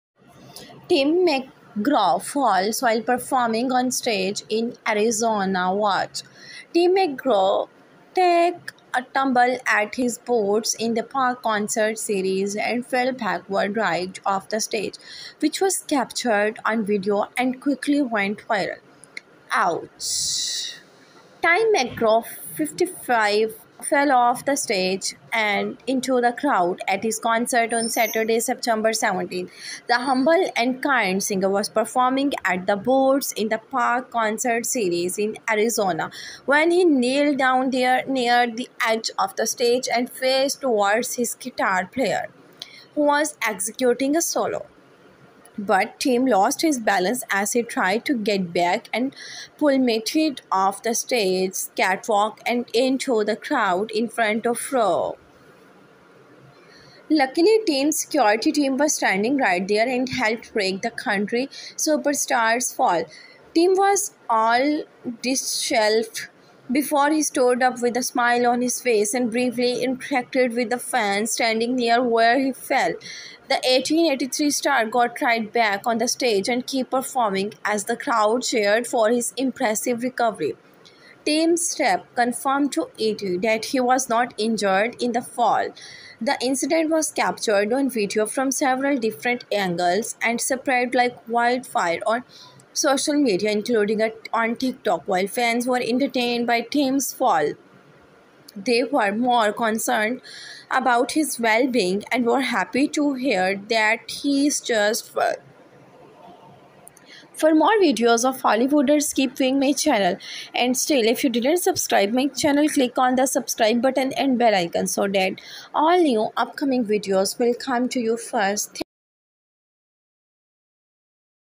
Tim McGraw Falls While Performing Onstage In Arizona: Watch
For More News of Hollywood keep watching my channel
Thankyou 😇